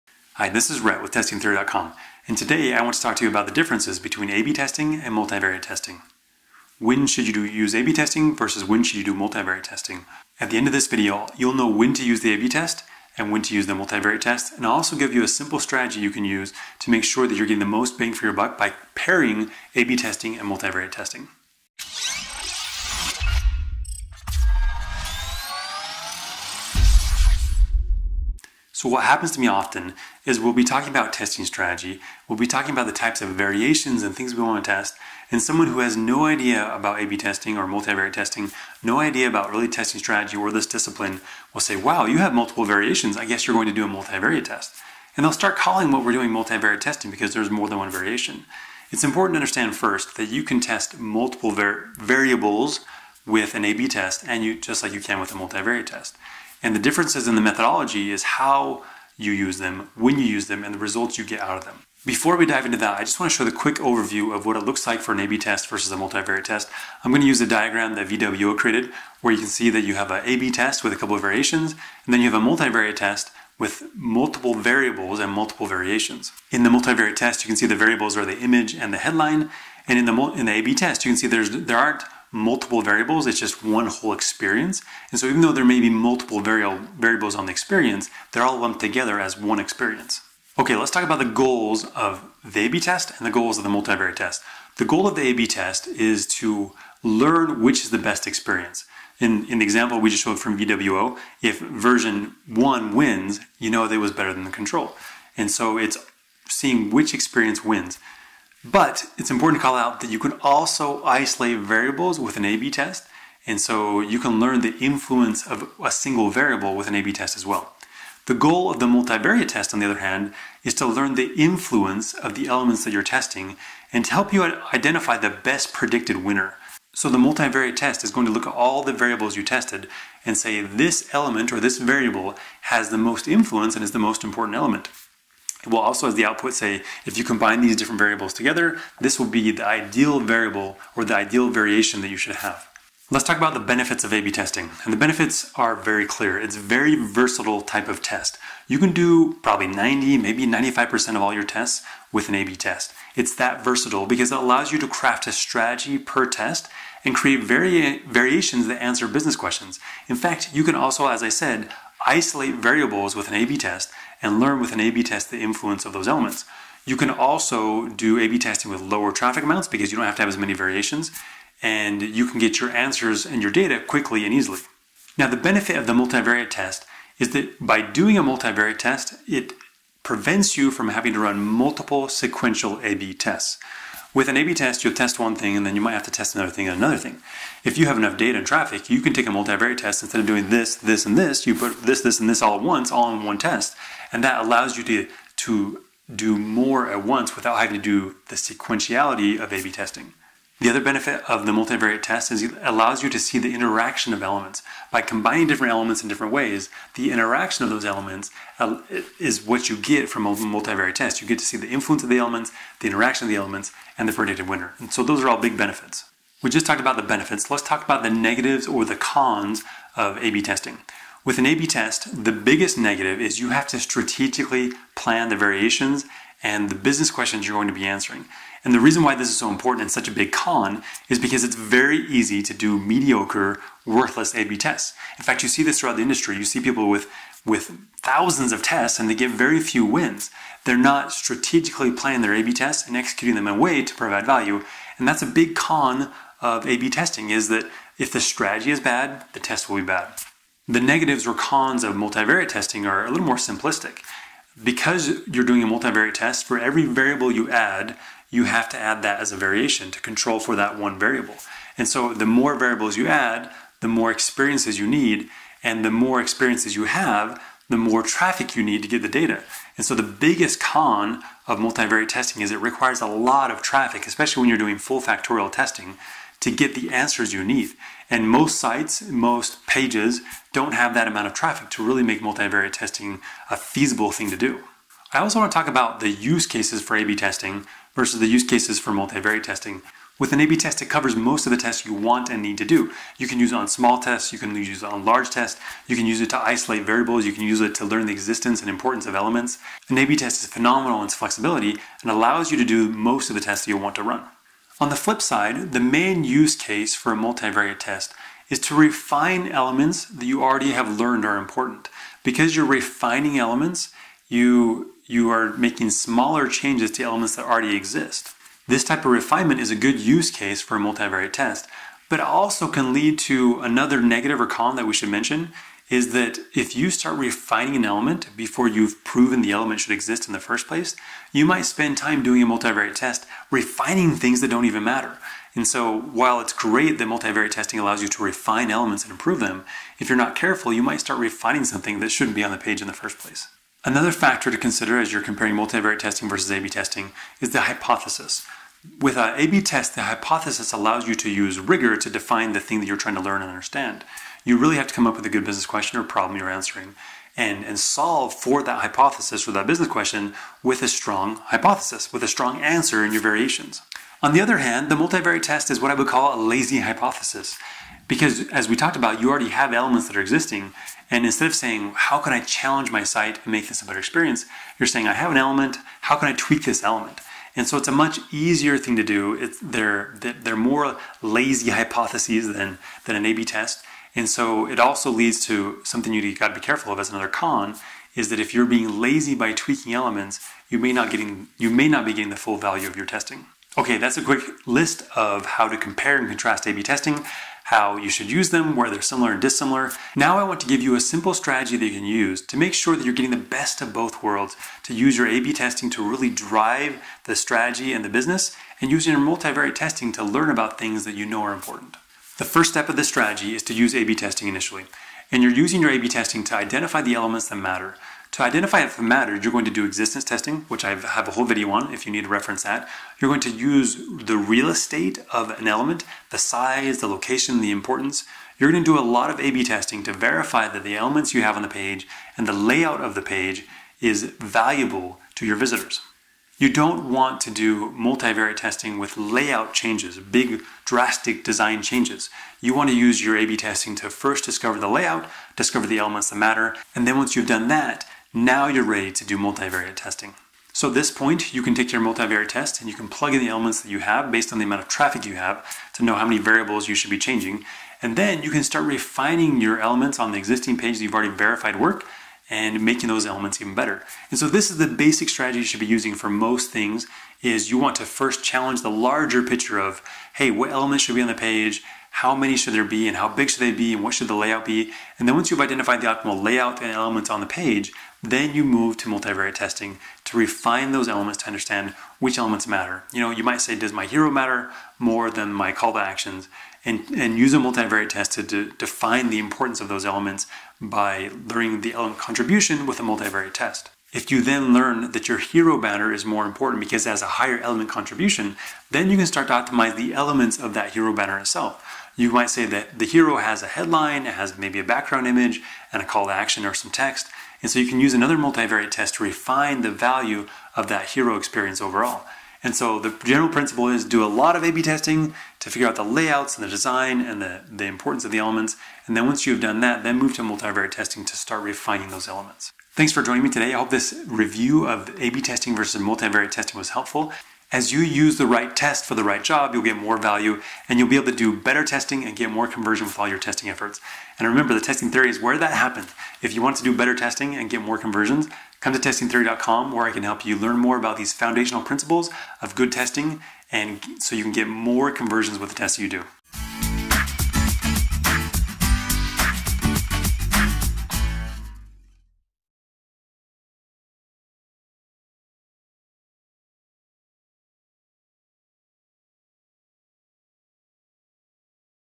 A/B Testing vs Multivariate Testing
What is the difference between an A/B test and a Multivariate test? When should you use one test versus the other? What are the key differences between multivariate testing and A/B testing? In this video, we will review the main points that conversion rate optimizers need to understand to appropriately leverage the right tool for the job.

I will also teach the holistic testing strategy of using A/B testing and multivariate testing together.

In this video, you will learn several things to improve your testing efforts including:
 • The Goals of the A/B test versus the multivariate test
 • The Benefits of doing A/B testing versus the multivariate testing
 • The Cons of the A/B test versus the multivariate test
 • The Use Cases for A/B testing versus the multivariate testing
 • How the hypothesis differs between the A/B test and the multivariate test
 • The Holistic Testing Strategy to leverage as you make the best use of both testing methods

Testing Theory is where professional testers turn to do better A/B testing and get more conversions.